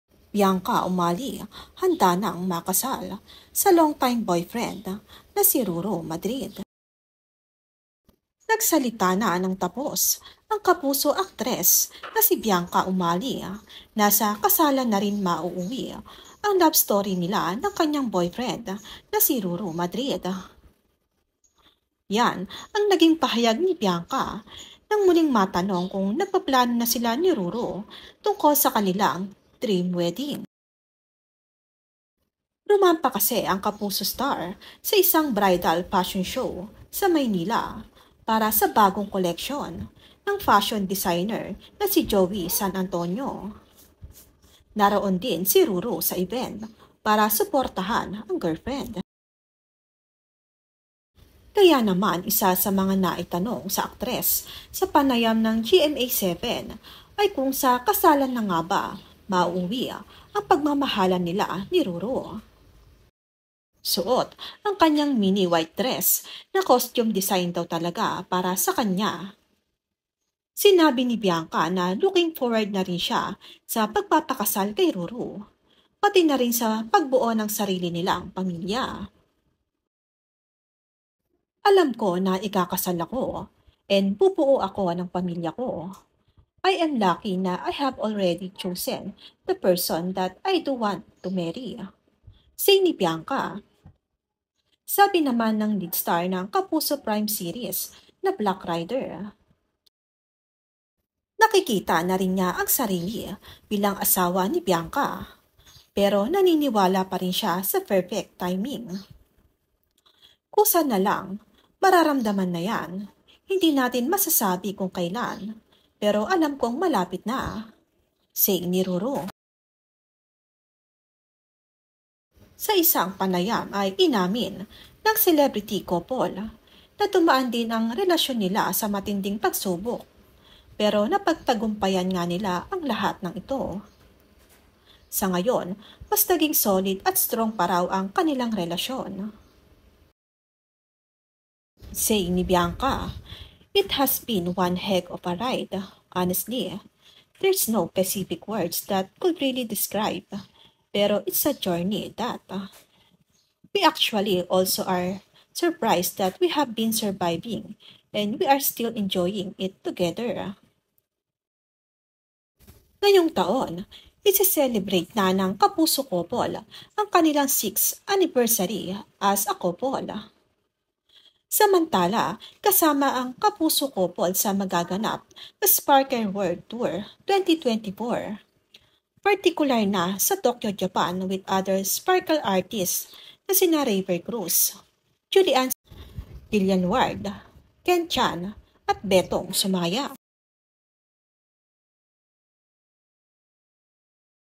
BIANCA UMALIS HANDA NA MAKASAL KAY RURU MADRID PLANO NA NGA BA NANG DALAWA MAGPAKASAL SUSUNOD TAON?
BIANCA UMALIS HANDA NA MAKASAL KAY RURU MADRID PLANO NA NGA BA NANG DALAWA MAGPAKASAL SA SUSUNOD NA TAON?